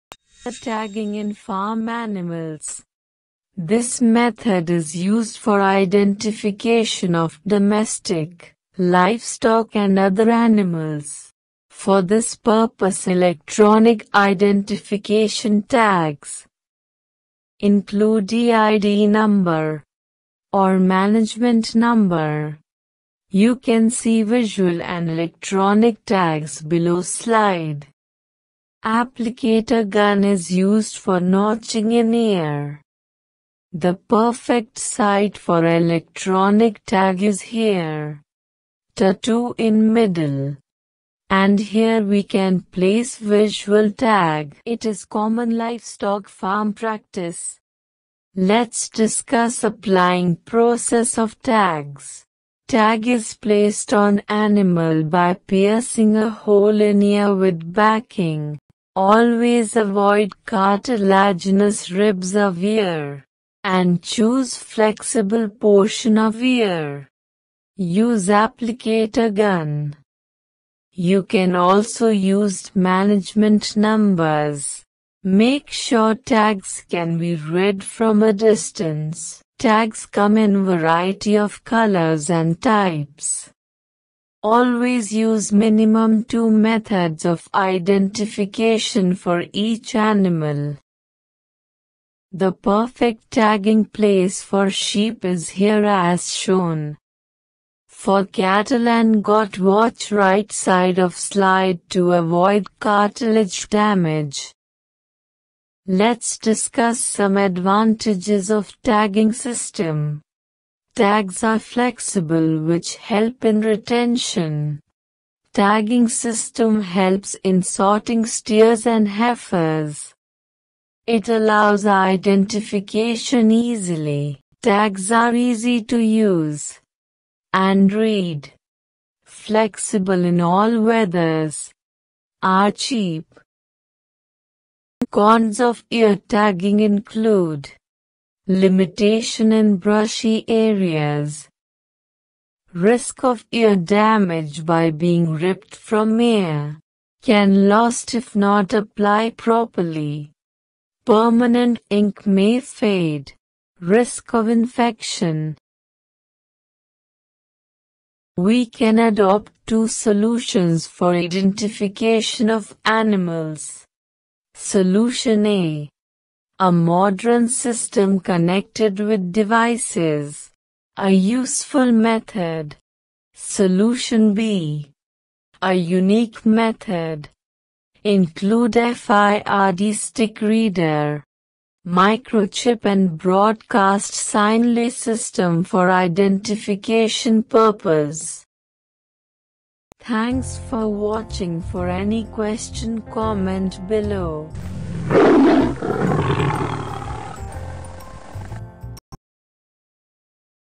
What is ear tagging in animals | Applying process | Cons & pros | Applicator gun
Ear taging of aninals in livestock farms
An ear tag is a plastic or metal object used for identification of domestic livestock and other animals. If the ear tag uses Radio Frequency Identification Device technology it is referred to as an electronic ear tag.Ear tags provide a visual aid for unique identification (such as a flock or premise number) and can help to identify sex, year of birth, sire, dam and much more. If you use tags in your breeding flock, it's wise to install a tag in both ears when they are baby lambs.
For more videos (related)
Ear tagging, Doctor of veterinary medicine